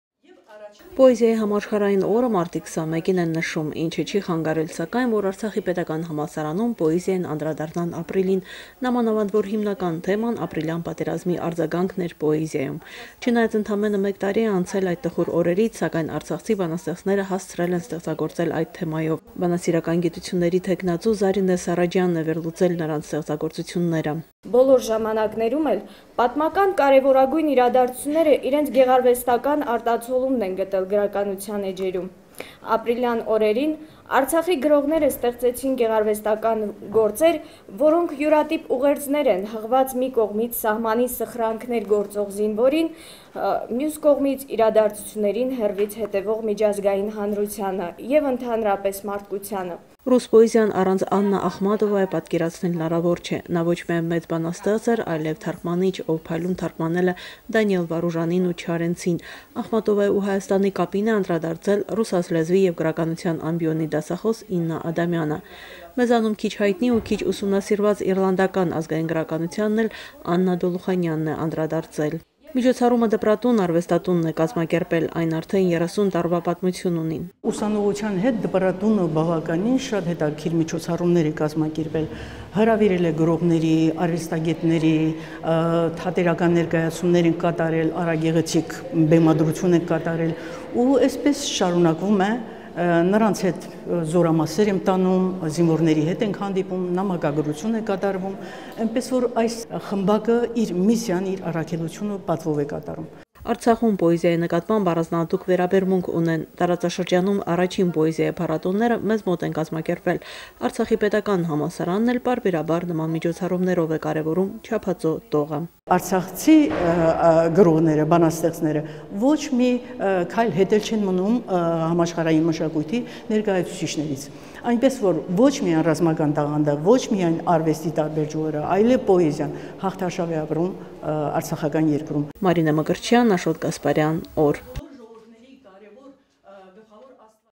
Արցախի պետական համալսարանում այսօր պոեզիայի օր էր
Արցախի պետական համալսարանում այսօր պոեզիայի օր էր: Կազմակերպիչը`   ̎Դպրատուն  ̎ արվեստատունն էր: Միջոցառմանը անդրադարձ է կատարվել ինչպես ժամանակակից արցախյան ու դասական պոեզիային, այնպես էլ  համաշխարհային նորույթներին:

Poetry Day in the ArSU
A Day of Poetry was held today in the Artsakh State University. The organizer of the event was the art house “Dpratoun”. The participants of the event presented the classical poetry, modern Artsakh poetry, as well as modern world poetry.